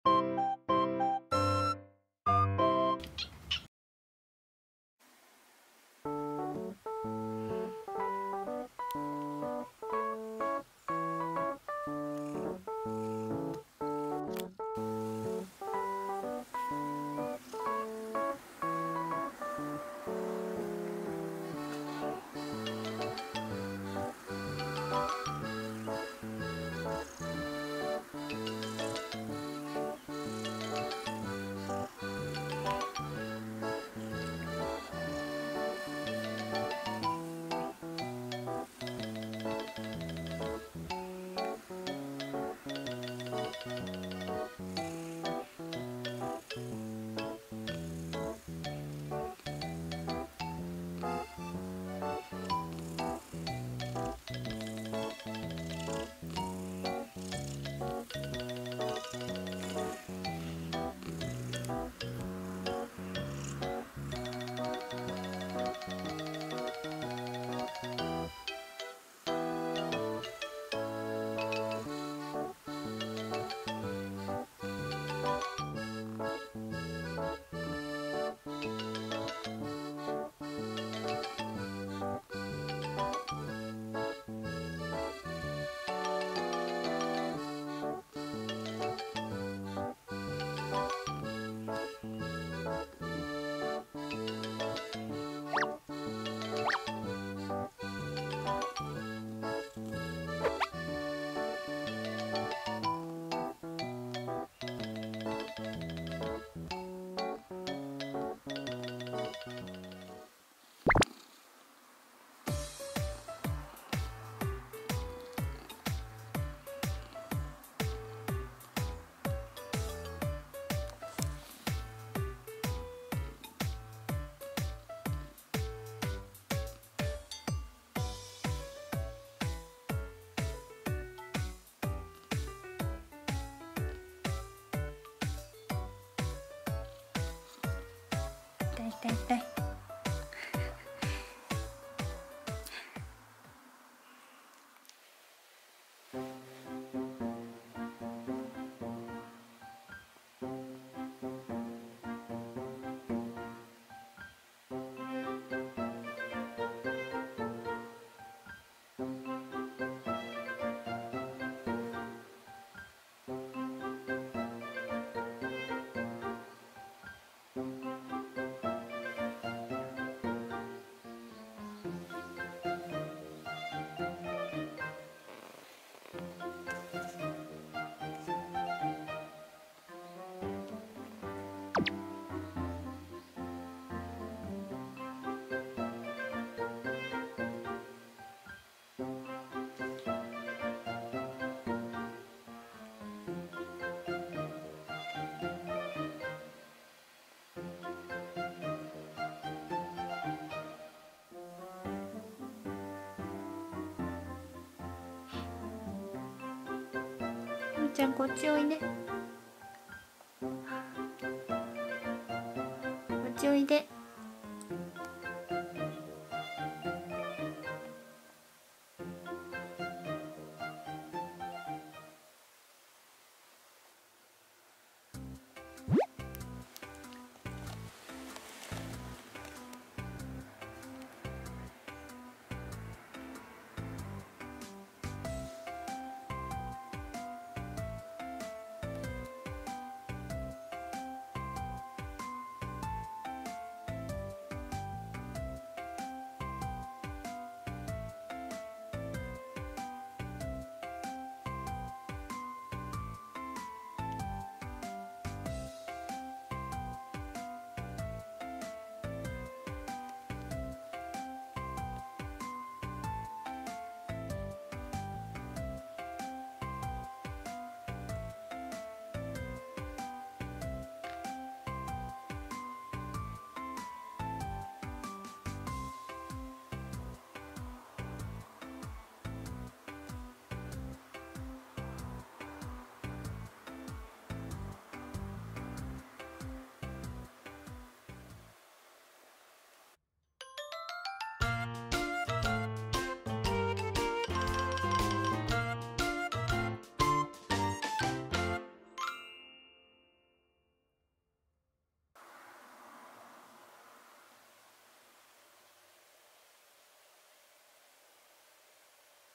遊んでもらえなくて拗ねちゃう短足マンチカンが可愛い！
短足マンチカンの男の子、プリンです！
プリンのかまって攻撃も、ひとりにしてっていう態度も結局可愛いんです！

質問や動画のリクエストはコメント欄にぜひお願いします！
☆ゆるっとサブチャンネル始めました！